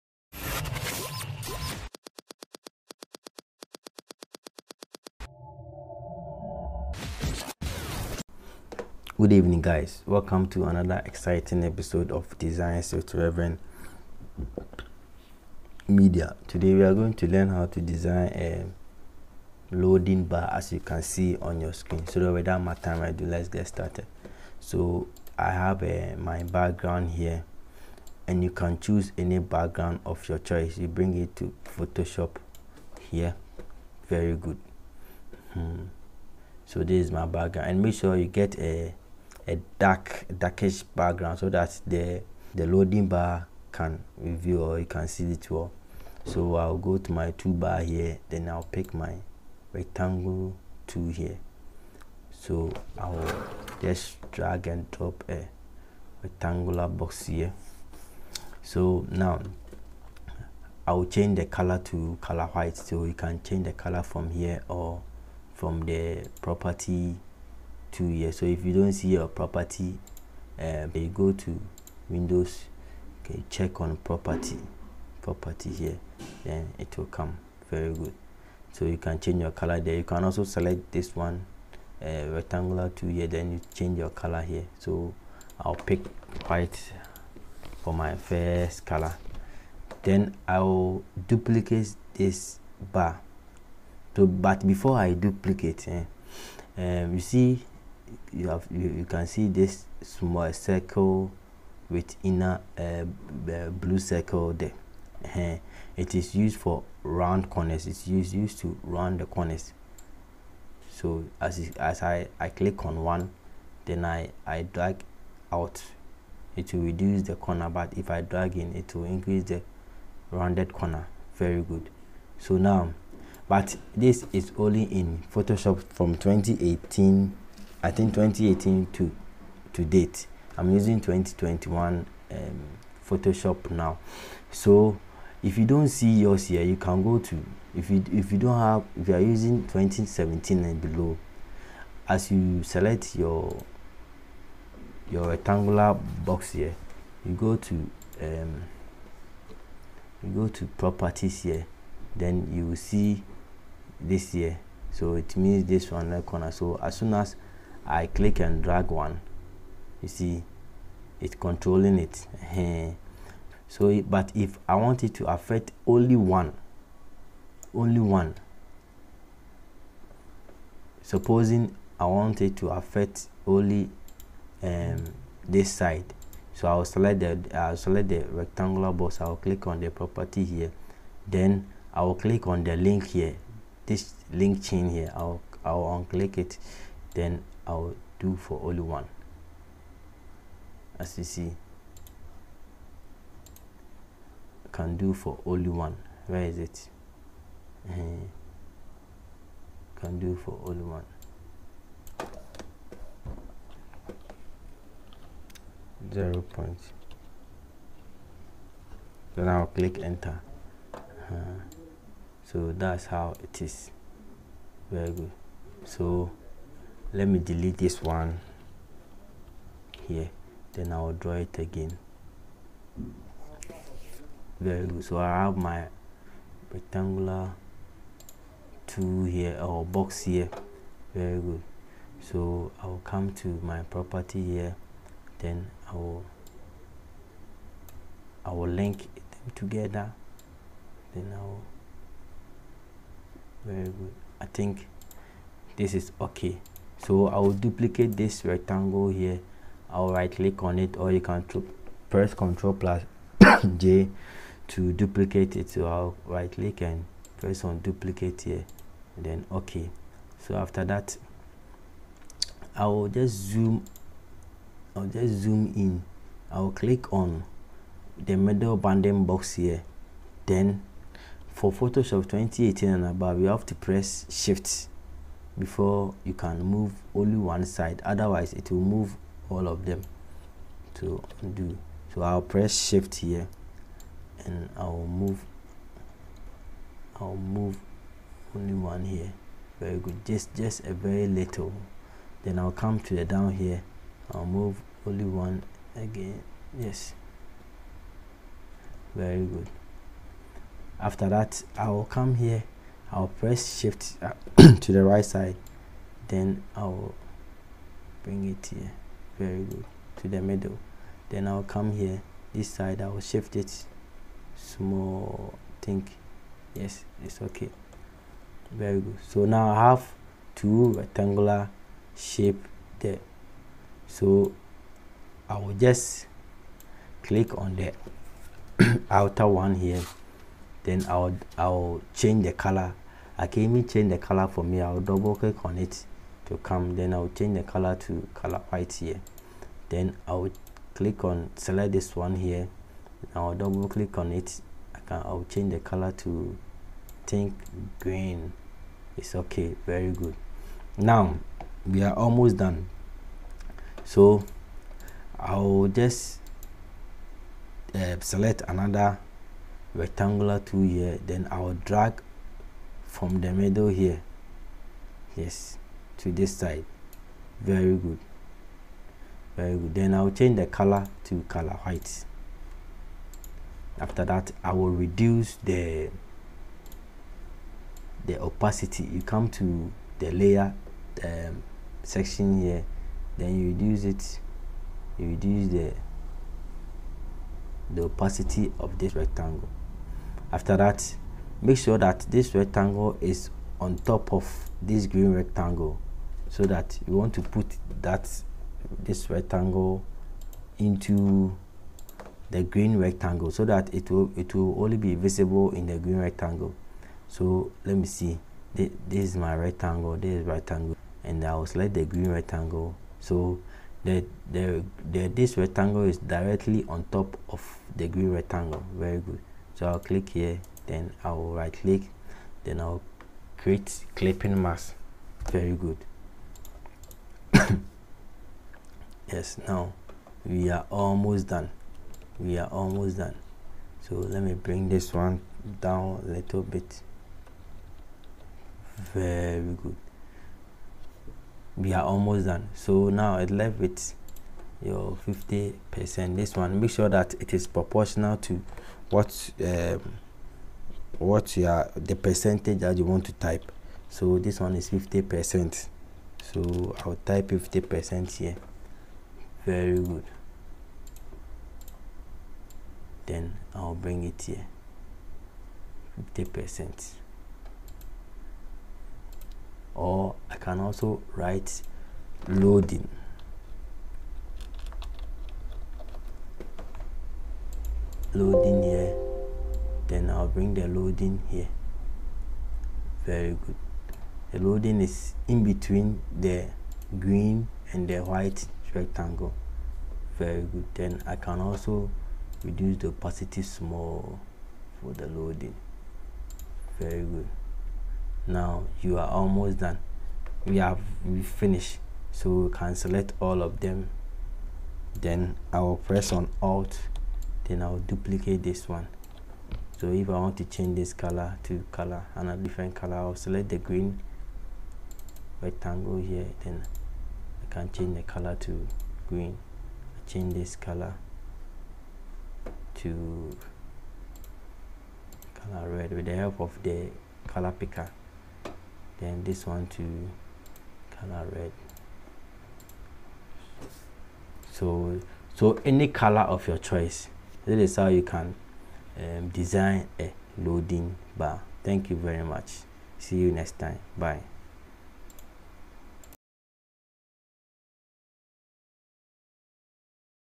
How to design a simple Loading Bar Photoshop CC tutorials 2021.
This tutorial is a step by step on how to design a simple Loading Bar. || Photoshop CC tutorials 2021.